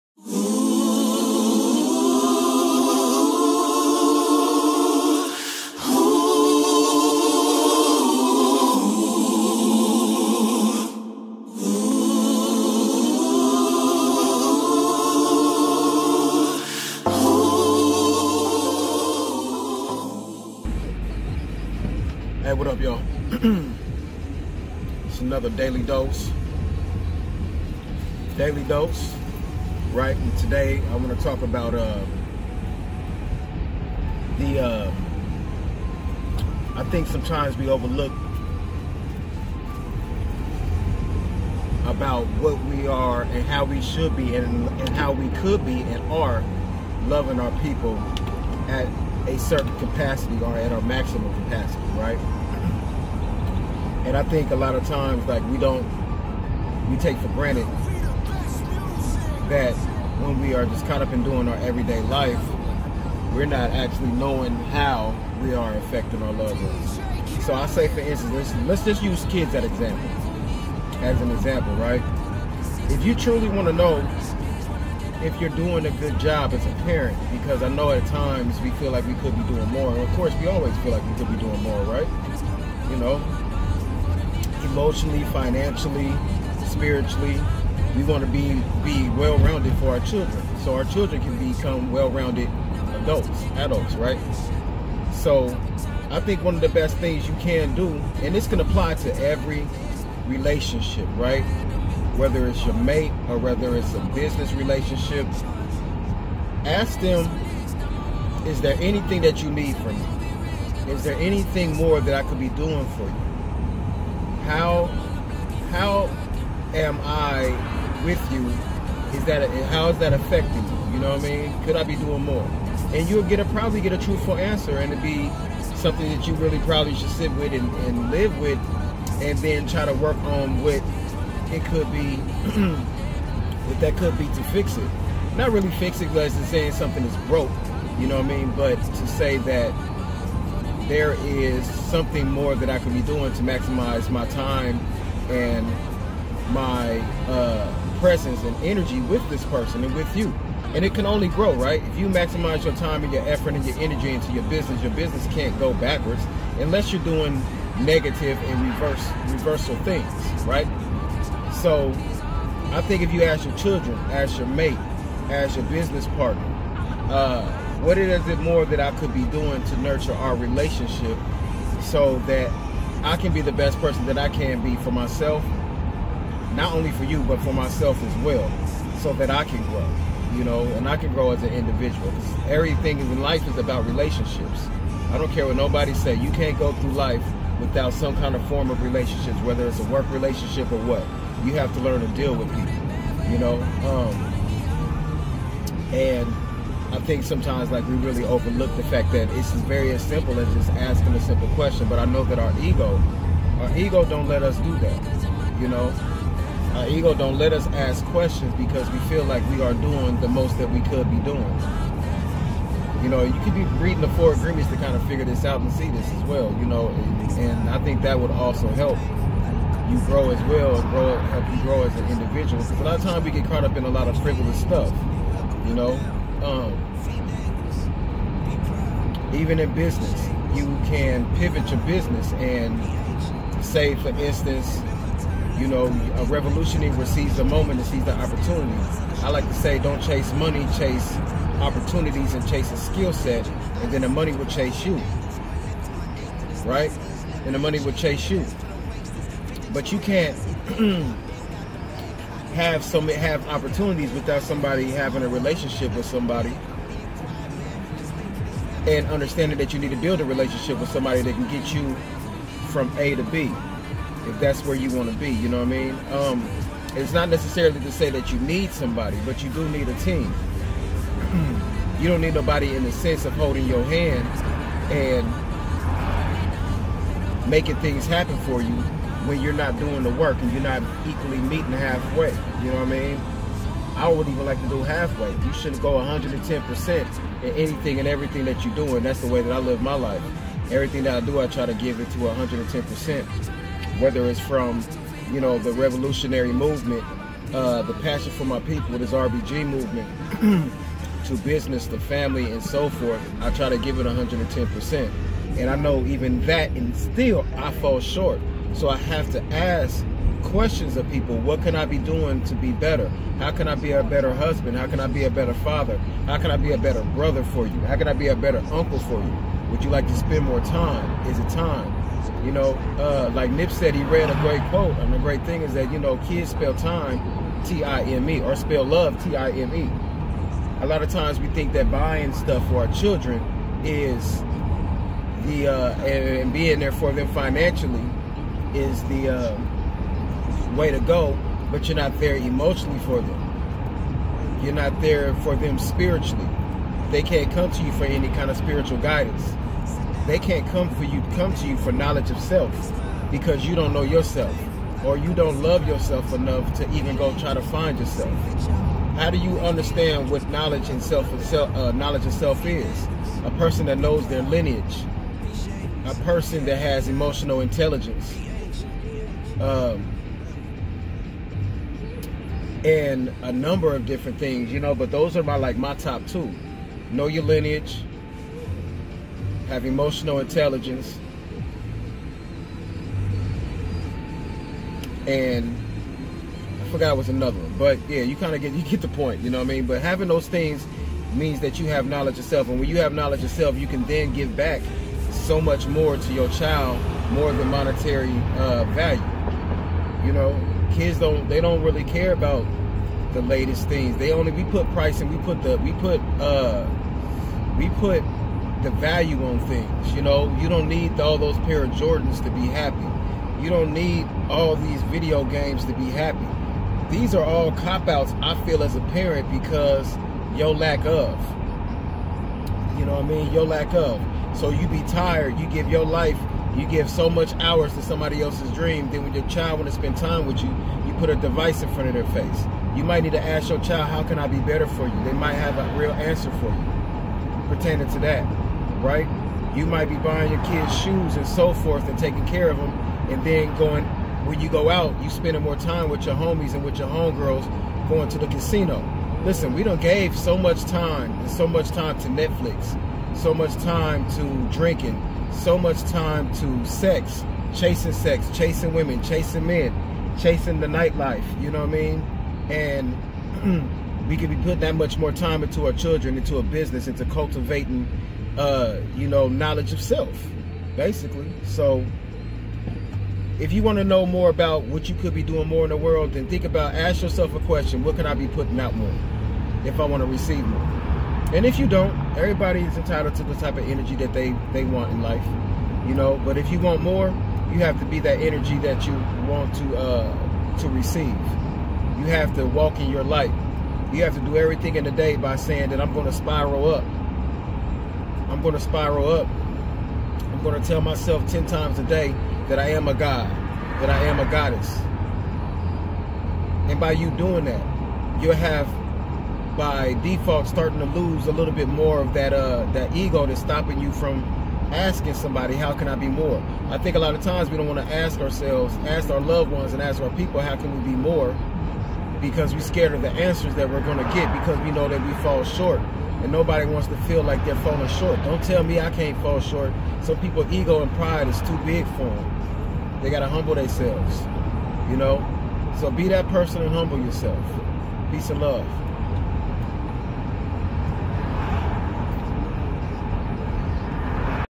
daily dose #1
🚨daily dose🚨

A lot of times well most of the time I believe we get in our own way of greatness by way of ego.
we could absolutely maximize our greatness potential simply by amplifying the most important voices around us.

too be a greater person ask what can I do more instead of what someone can do for me.

💉know ones lineage
💉Emotional intelligence
💉stand on one's square